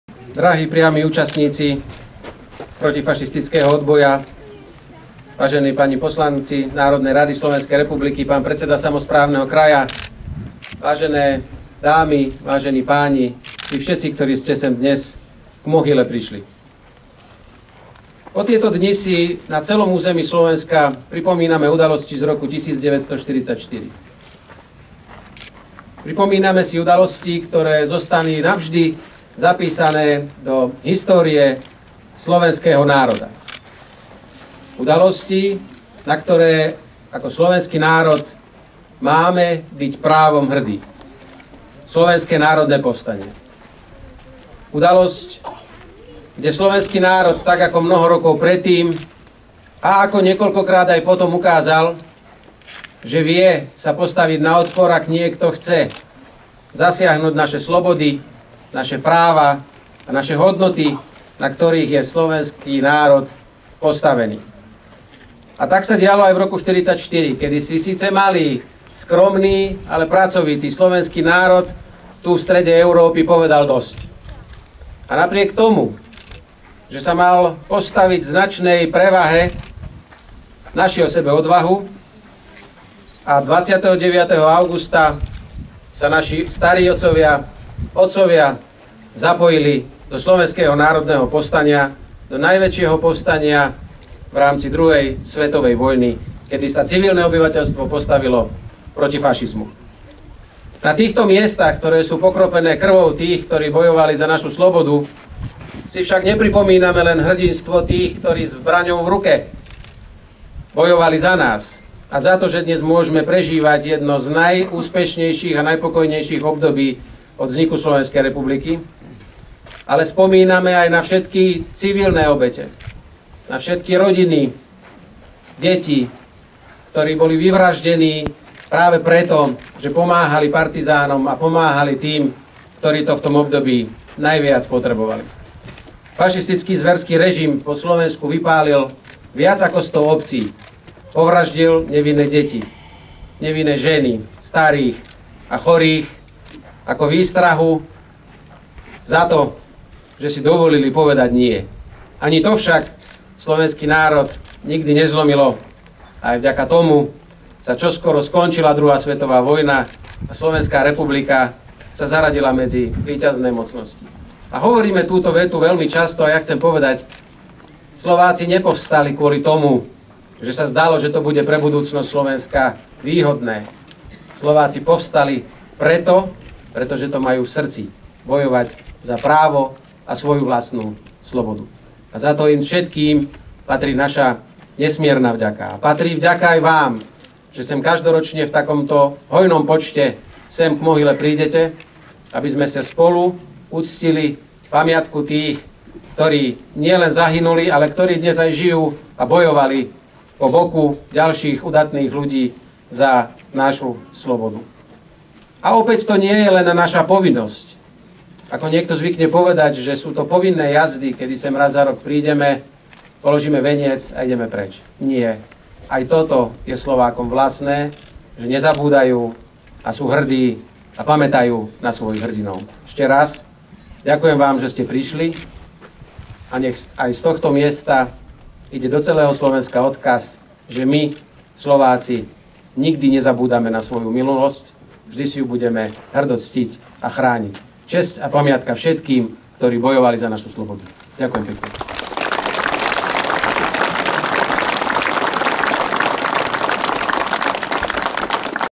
P - Príhovor premiéra P. Pellegriniho - pri podujatí 74. výročia SNP - Jankov vŕšok - 25.08.2018
TK  Tlačová konferencia
TB  Tlačový brífing
P     Príhovor
I      Ilustračný záznam